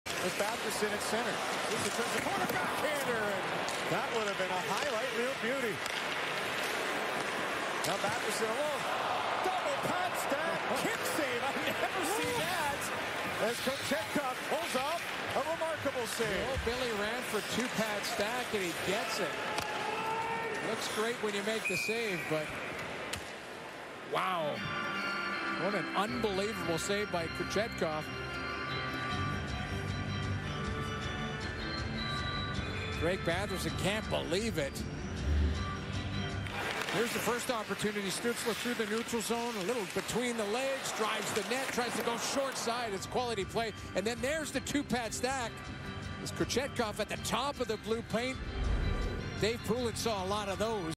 Pyotr Kochetkov's old school pad save on Batherson vs Senators (12 dec 2023)
Классный сэйв Петра Кочеткова в игре с "Оттавой".